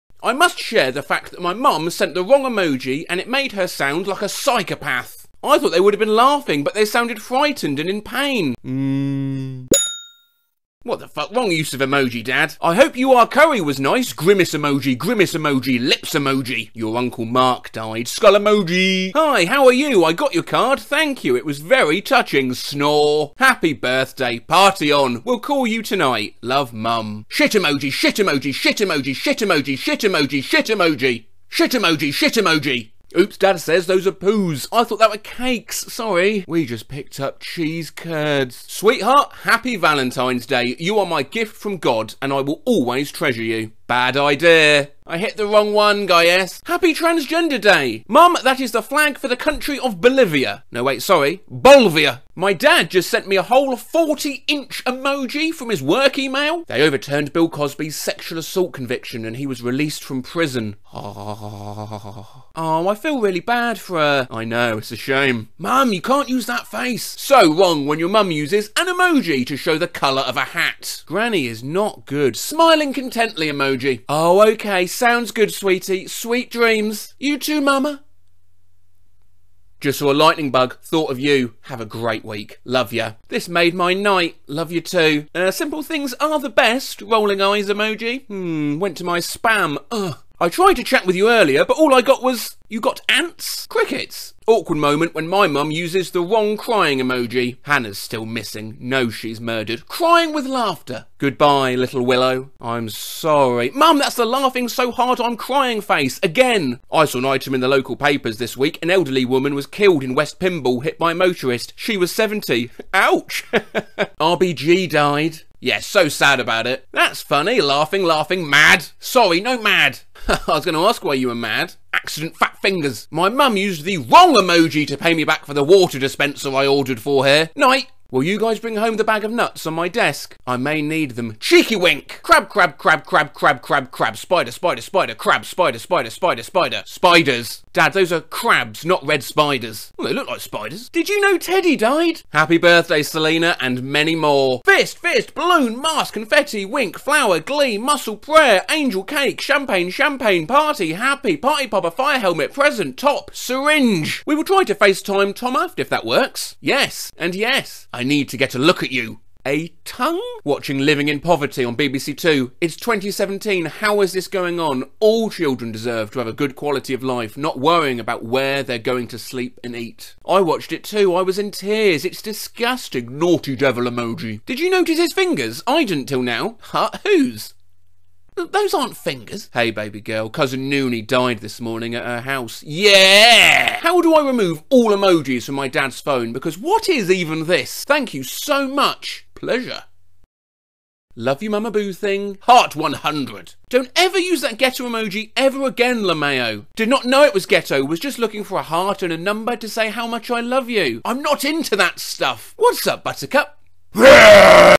Parents doing emojis wrong
The humble by-product of searching Twitter for the words "parents", "wrong" and "emoji". Hope you enjoy the video, from Uncle Matt, LOL (Lots Of Love)